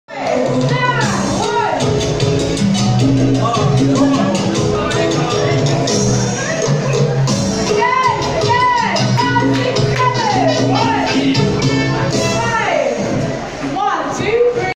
Drop in Salsa and Bachata with Amanda Hawley
Every week we run drop in classes for all levels at Revolucion De Cuba in Nottingham on Wednesdays. this is a sample of some of the stuff that you could be learning. to find out more.